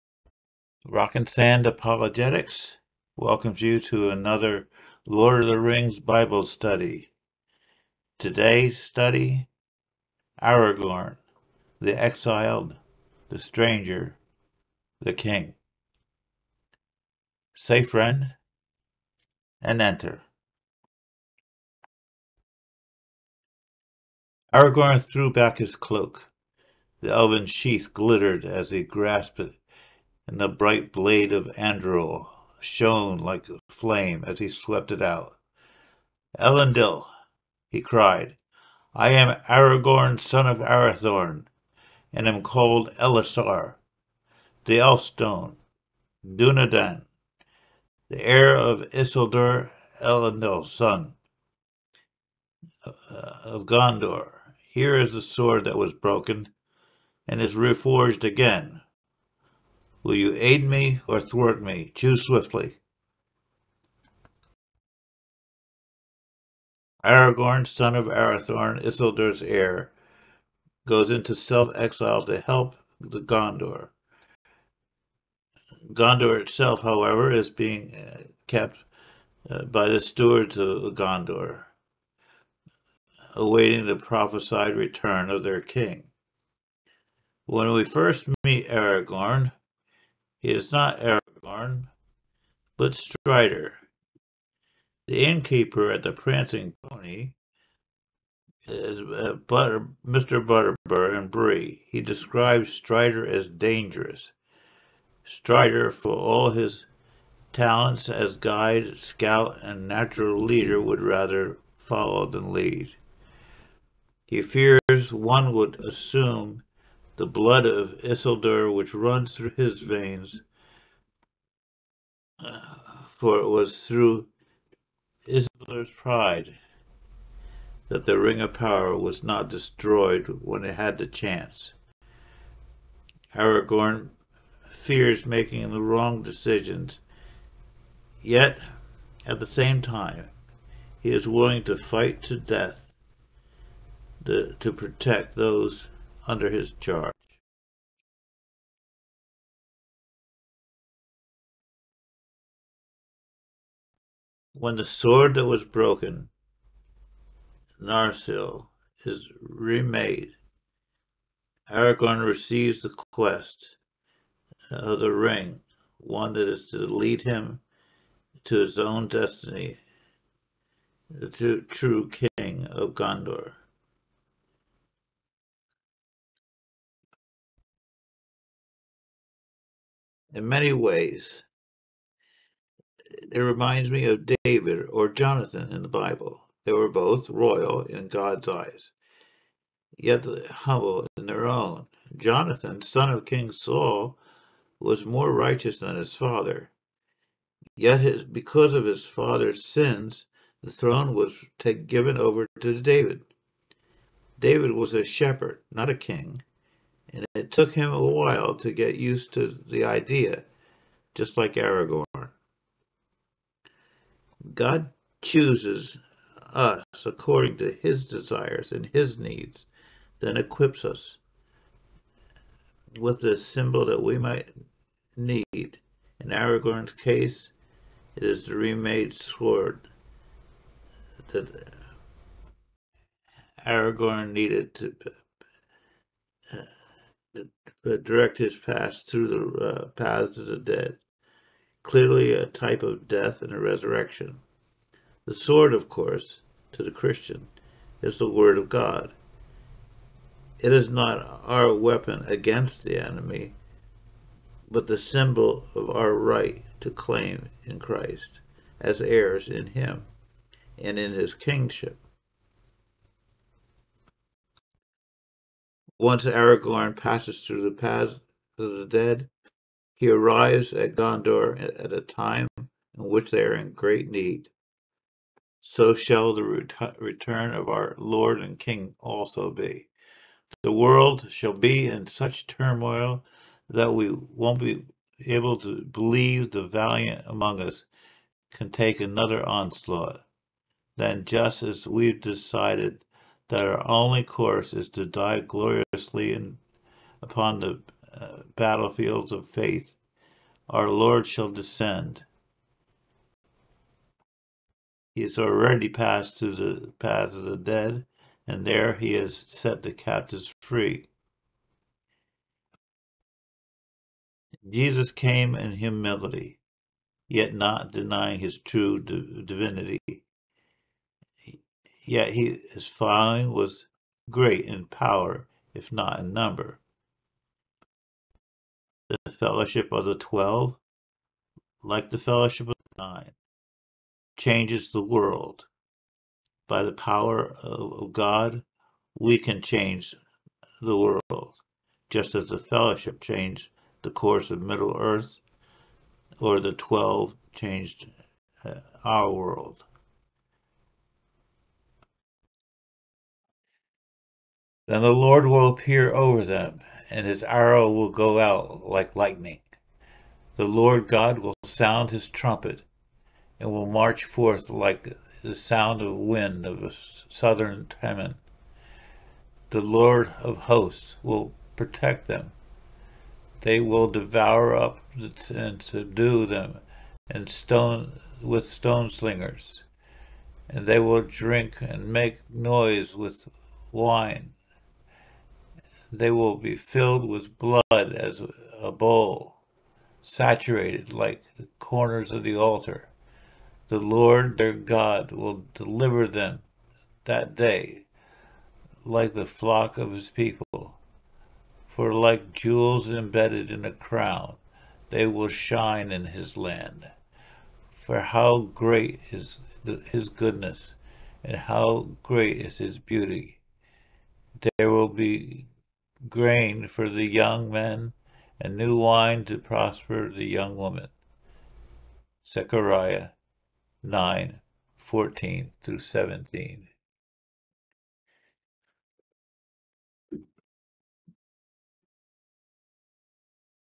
7. He was afraid to be king
Lord of the Rings Bible Study Lesson 5:
Aragorn: The Exiled, The Stranger, The King  Here we make comparisons with Aragorn and people in the Bible, the most prominent being Jesus, the King of kings.